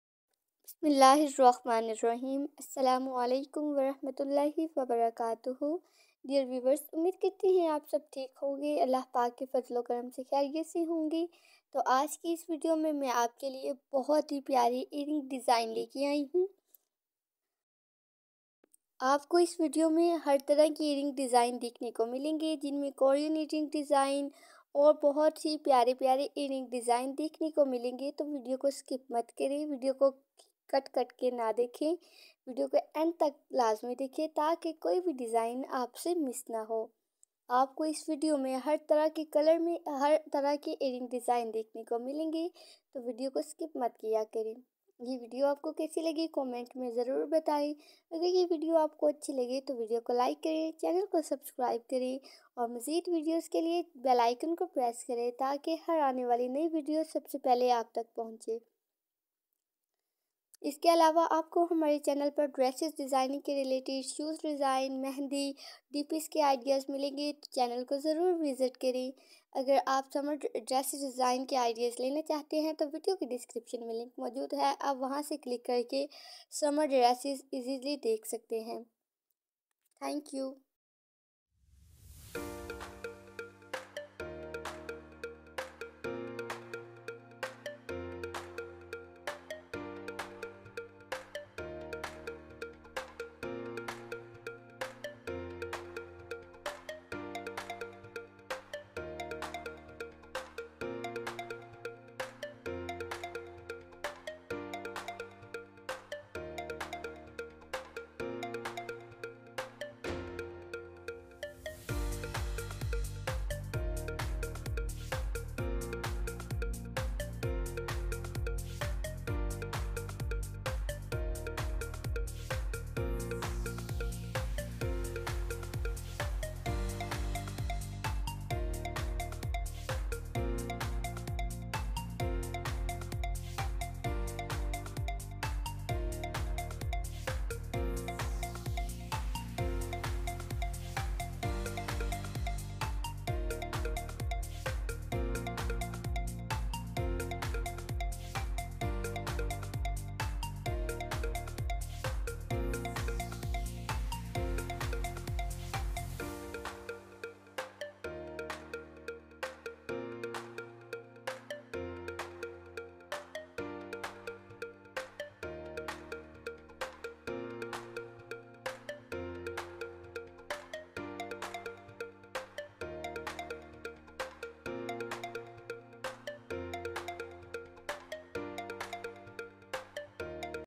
🔥🔥🔥Every type of earring design 🥰/for party/for wedding/daily wear earrings design
Assalam Alaikum today in this vedio I want to share with you every type of earring design for girls

searching keywords

artificial beautiful earing stylish and attractive design every type of earing
how to make earing at home
earing art ideas

jazakallah